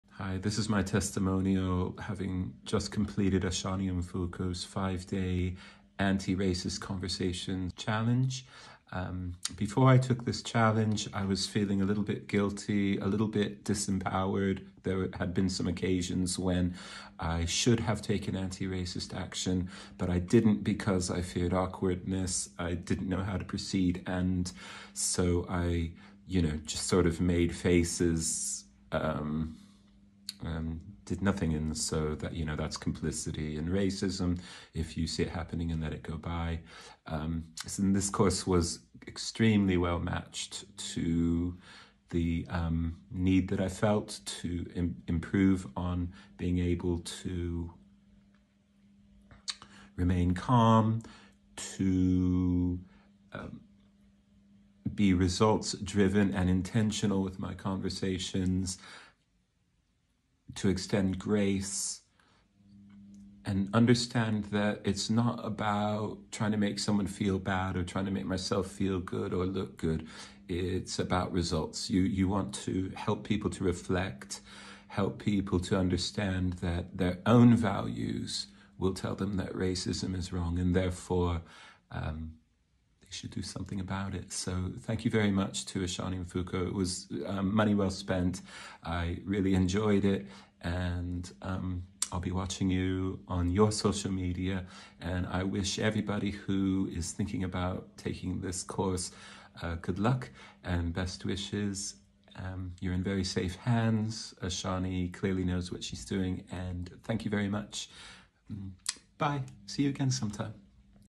ASHANI MFUKO ANTI-RACISM EDUCATOR STUDENT TESTIMONIAL, ANTI-RACISM COURSE, ANTI-RACISM CHALLENGE
Ashani Mfuko, Anti-Racism Educator, 5-Day Anti-Racist Conversations Confidence Challenge Student Testimonial.

Thank you Andy!☺️🙌🏽